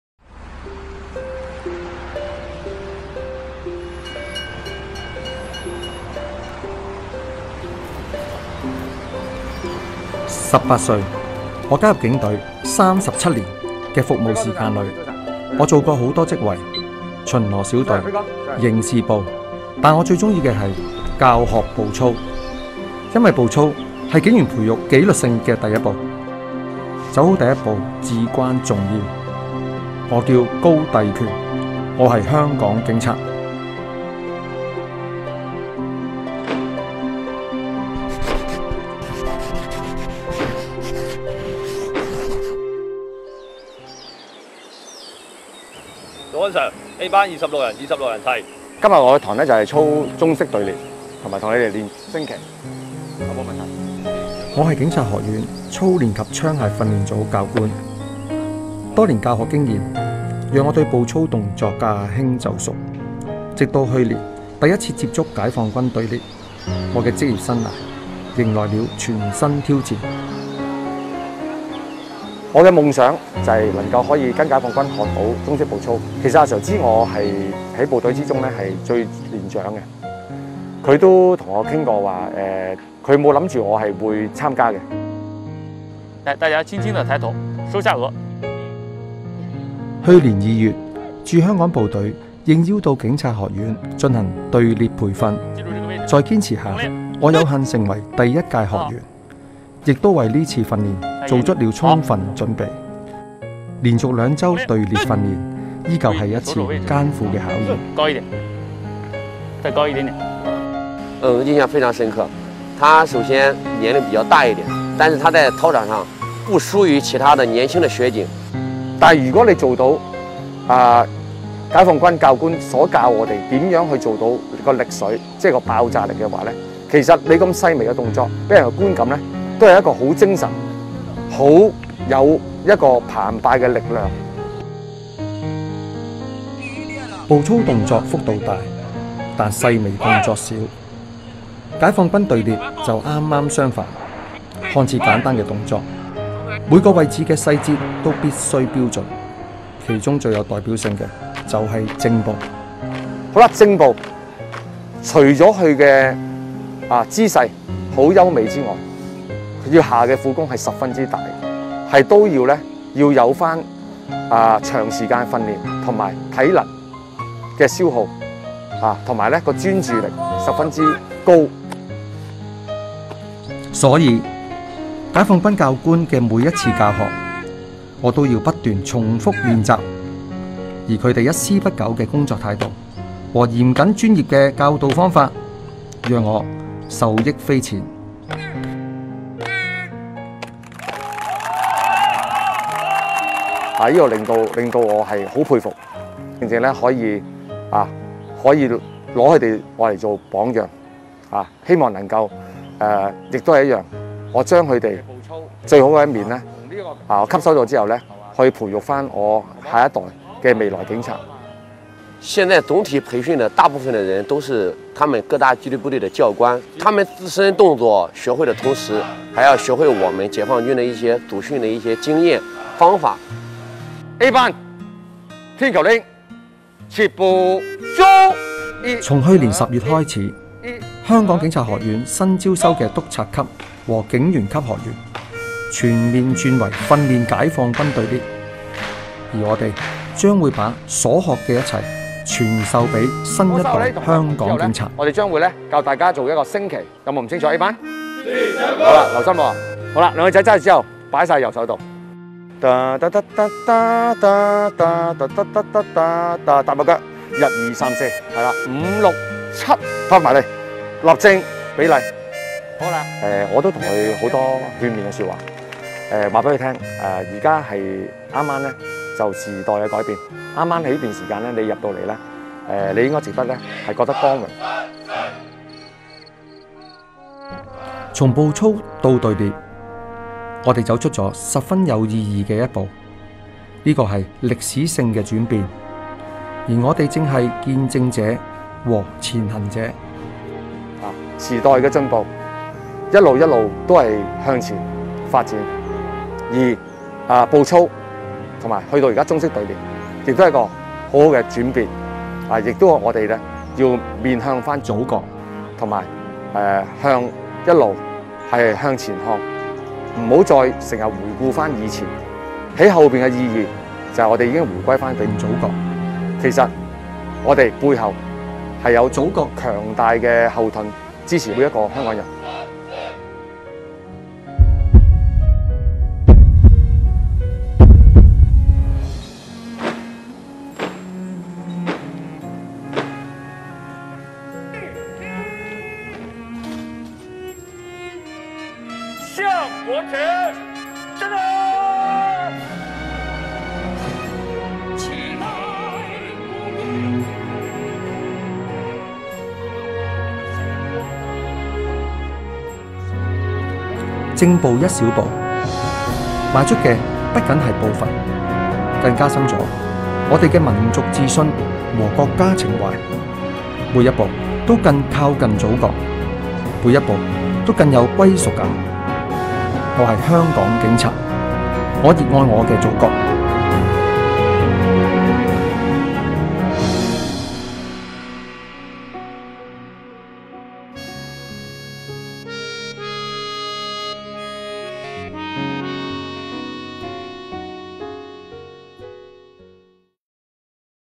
香港警长的中式步操：正步走，每一步都更靠近祖国
2021年开始，驻香港部队应邀为香港纪律部队组织解放军队列培训。许多香港纪律部队参训人员，在与解放军教官的相处过程中教学相长，结下深厚的友谊，香港警察高帝权就是其中一位。他说，从步操到队列，走出了十分有意义的一步；每一步都更靠近祖国，每一步都更有归属感。